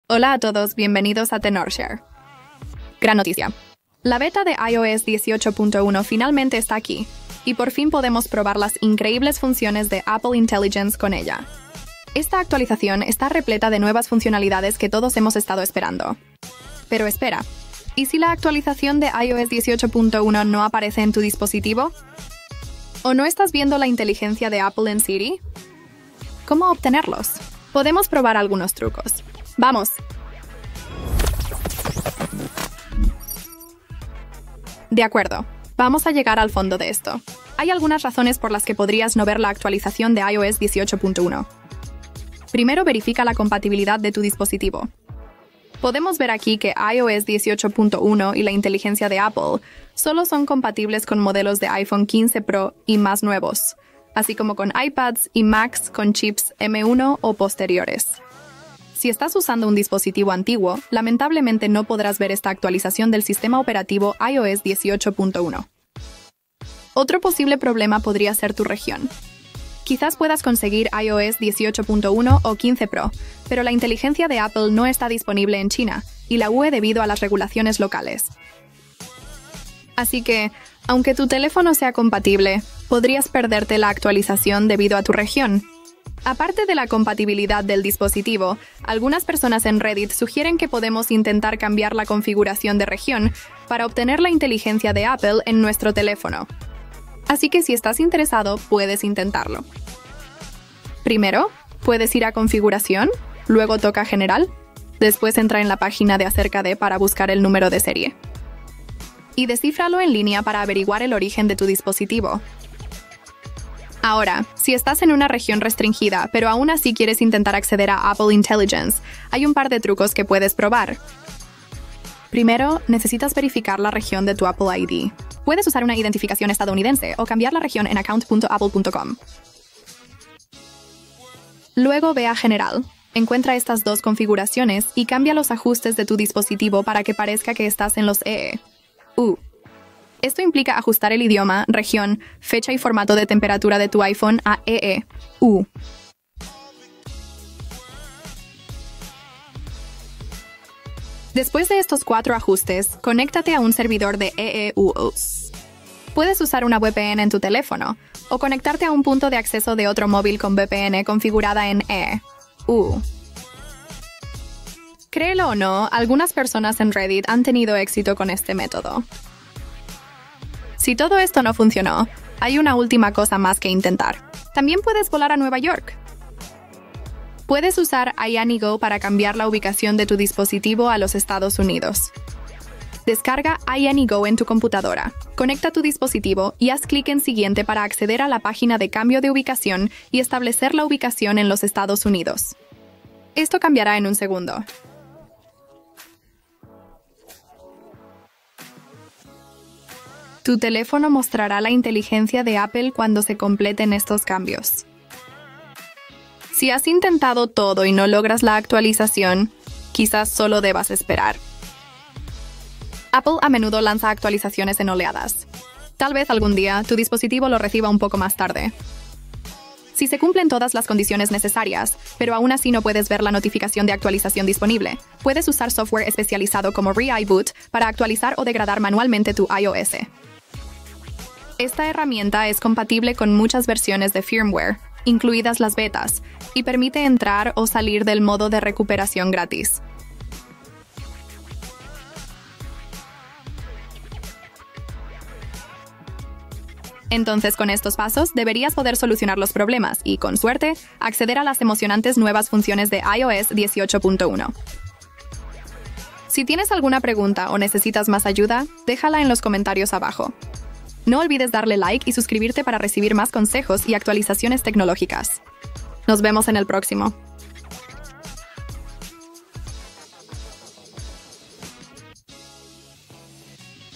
¿iOS 18.1 NO Muestra? ¡Unirse a la lista de espera de Apple Intelligence fuera de EE.UU.!
🔍 ¿No ves la actualización de iOS 18.1 en la UE? ¿Apple Intelligence aún no está disponible? 🚀
¿Te mueres por probar las últimas características de iOS 18.1 y Apple Intelligence, pero no puedes acceder a la actualización en Europa? ¡No te preocupes! Hemos reunido los mejores consejos para ayudarte a solucionar este problema y asegurarte de que tu dispositivo esté al día con la versión más reciente.

En este video descubrirás:
✨ Cómo encontrar y acceder a la actualización de iOS 18.1 si no aparece en tu dispositivo.
🔧 Soluciones rápidas para problemas comunes relacionados con Apple Intelligence en Europa.
📲 Métodos alternativos para actualizar manualmente tu teléfono y disfrutar de todas las nuevas funciones.

No te pierdas ninguna novedad y mantén tu dispositivo en su mejor estado. ¡Dale al play y sigue nuestros consejos para estar siempre a la vanguardia con iOS 18.1!

⏱Marca de tiempo:
00:00 Introducción
00:30 ¿Por qué no aparece iOS 18.1 / Inteligencia de Apple? (Inteligencia de Apple no está disponible)
 Get Around Region Restrictions(Únete a la lista de espera de Inteligencia de Apple fuera de la región de EE.UU.)
01:30 Paso 1: Comprobar la región del dispositivo
01:55 Paso 2: Cambiar la región del ID de Apple
02:10 Paso 3: Cambiar los Ajustes a «Modo EEUU
02:30 Paso 4: Conectarse al servidor de EE.UU.
02:50 Paso 5: Usar un Cambiador de Ubicación
03:40 Método 3: Usar ReiBoot para obtener iOS 18.1

📌¿Por qué no aparece iOS 18.1 / Inteligencia de Apple?
● Compatibilidad de dispositivos: iOS 18.1 y Apple Intelligence solo son compatibles con los modelos iPhone 15 Pro y posteriores, y los iPad y Mac con chips M1 o posteriores. Los dispositivos más antiguos no son compatibles.
● Restricciones regionales: Apple Intelligence no está disponible en China y la UE debido a las regulaciones locales.
📌Cómo evitar las restricciones regionales
●Paso 1: Comprueba la región del dispositivo : Ve a Ajustes - General - Acerca de - Número de serie para averiguar el origen de tu dispositivo.
Paso 2: Cambia la región de tu ID de Apple:  Cambia la región de tu ID de Apple en account.apple.com.
-●Paso 3: Cambia los Ajustes a «Modo EE.UU» : Ve a Ajustes - General - Idioma y Región - ajústalo a EE.UU.
●Paso 4: Conectarse al servidor de EE.UU.: Utiliza una VPN para conectarte a un servidor de Estados Unidos.
●Paso 5: Usa un Cambiador de Ubicación:  Usa iAnyGo para cambiar la ubicación de tu dispositivo a Estados Unidos.
📌Método 3: Usa ReiBoot para obtener iOS 18.1
● Utiliza ReiBoot para actualizar o downgradear manualmente tu iOS.
● Soporta una amplia gama de firmware, incluyendo las últimas versiones beta.
● Ayuda a entrar o salir del modo de recuperación de forma gratuita.